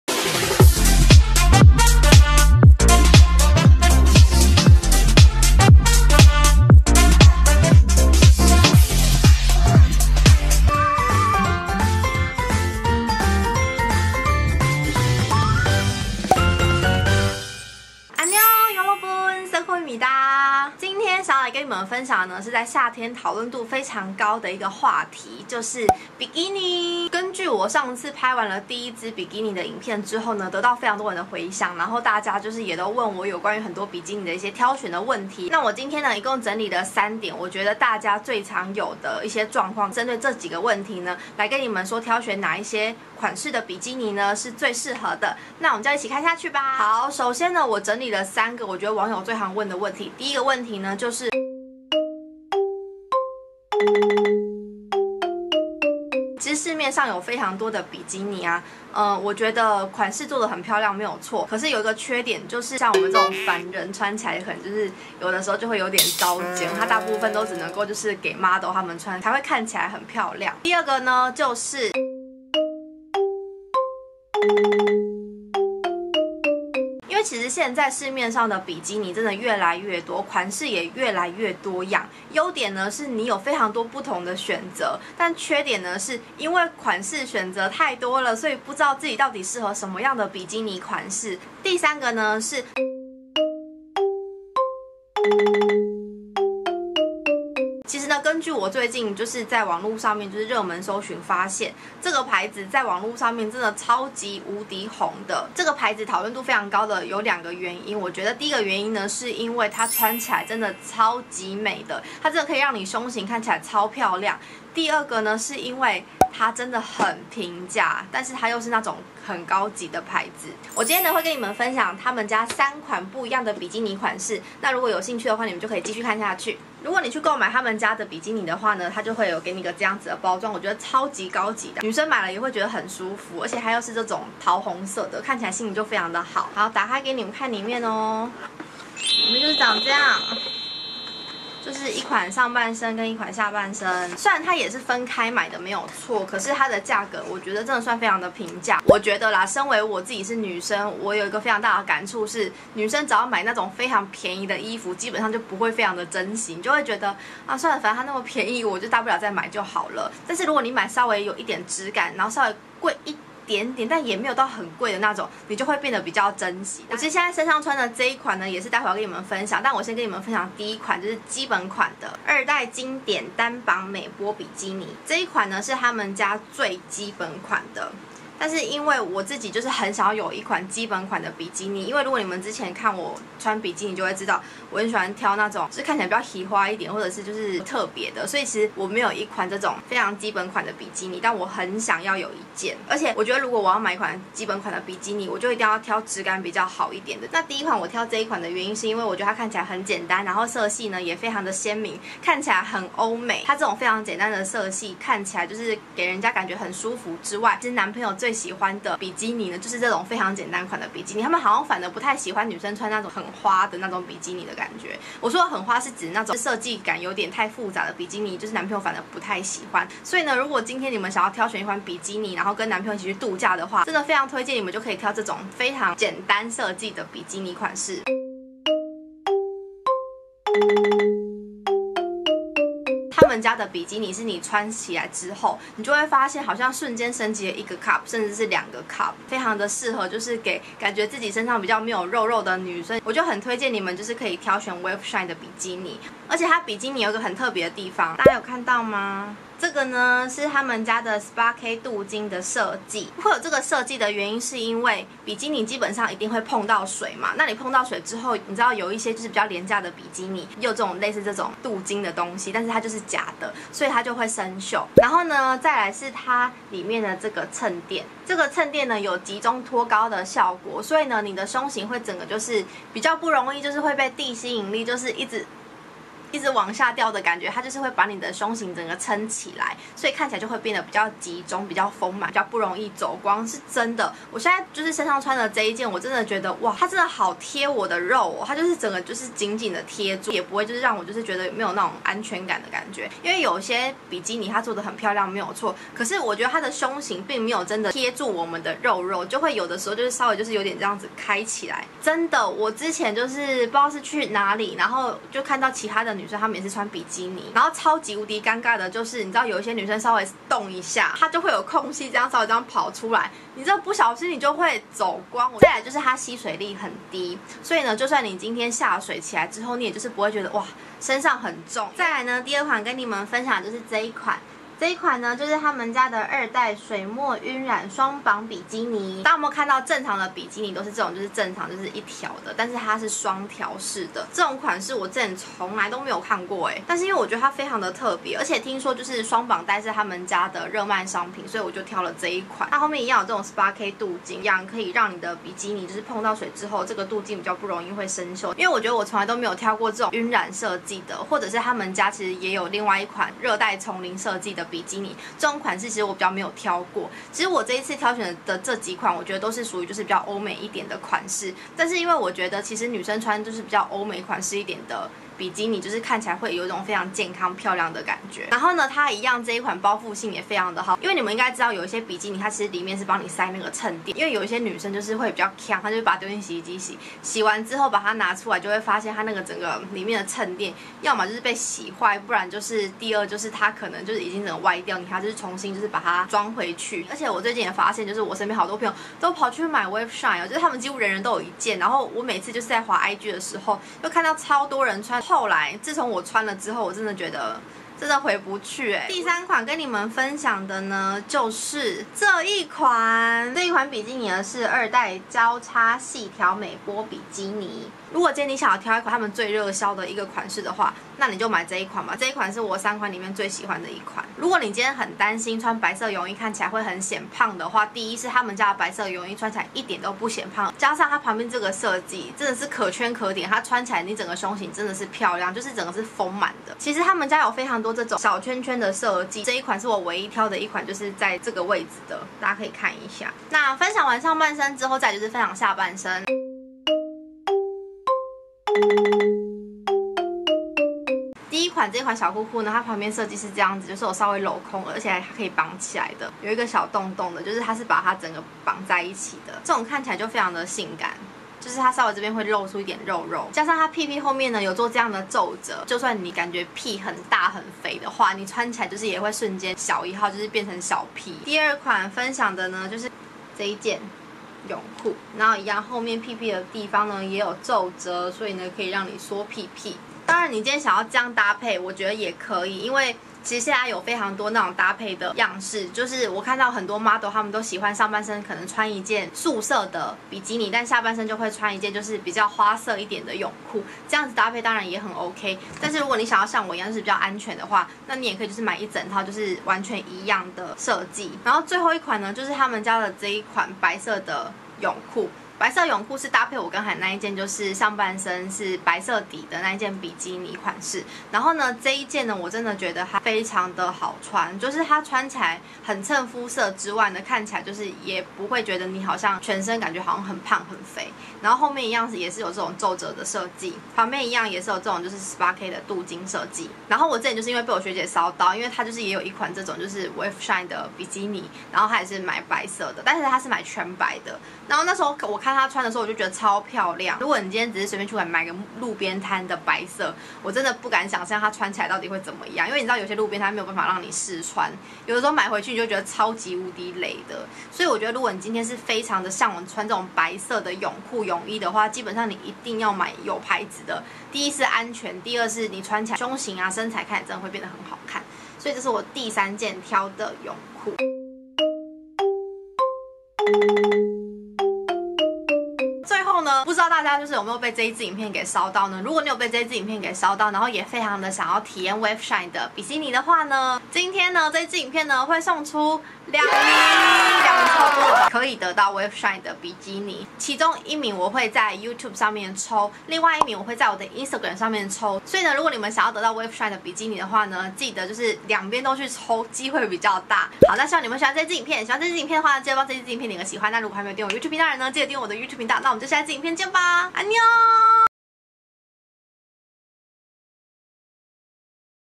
【穿搭】一起變身”泳池邊的小惡魔❤️👿“（抽獎） feat. wave shine平價美波神器
安妞❤️ 這次抽獎的活動辦法在這邊唷～

抽獎辦法：
請大家去wave shine官網，找到一件自己最喜歡的比基尼款式，把名字記下來，分享在留言板下方！
（記得，要誠實～不可以複製貼上別人的答案喔！！）

當然如果你有什麼想說的話，也可以留言跟我說❤️

Youtube這邊會抽出一位❤️
Instagram也會抽出一位唷❤️
抽到的圓仔們，我會直接私訊或寄信給你💗

***只抽圓仔唷，所以不是阿圓讀者的sorry**
因為這是回饋給圓仔們的小禮物🎁

莎朗嘿❤️

膚況：混合肌偏乾，額頭與下巴容易出油，但臉頰偏乾
使用相機：PANASONIC GF8
影片剪輯軟體：FINAL CUT PRO
封面製作軟體：Photoshop